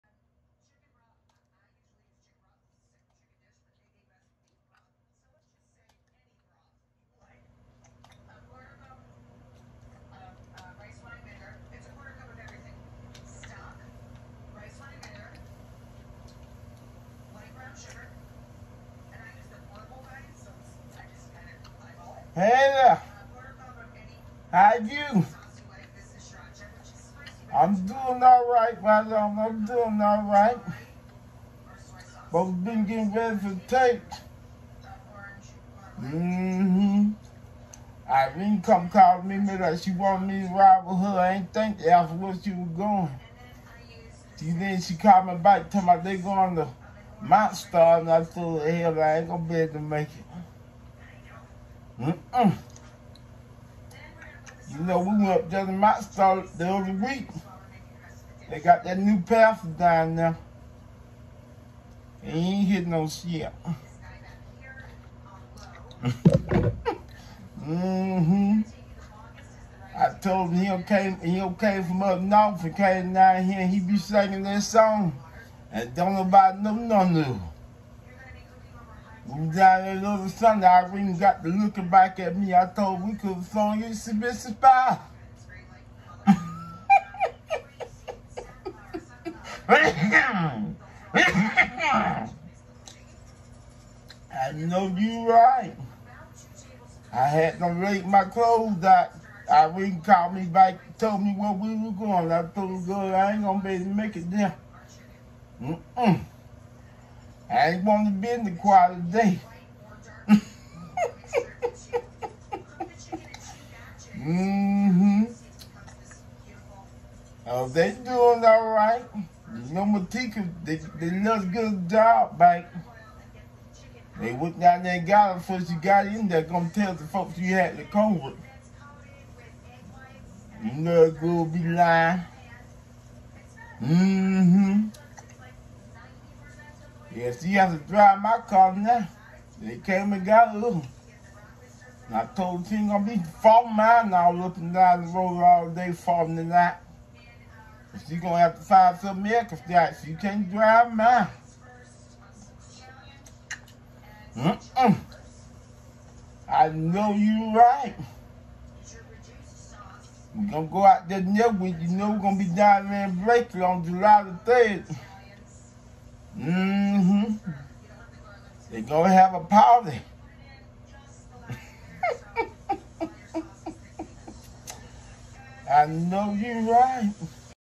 Ms.Bert was not going to Mt.Star!
Irene wanted Ms.Bert to go to Mt.Star and she was not having it! Find out what happen!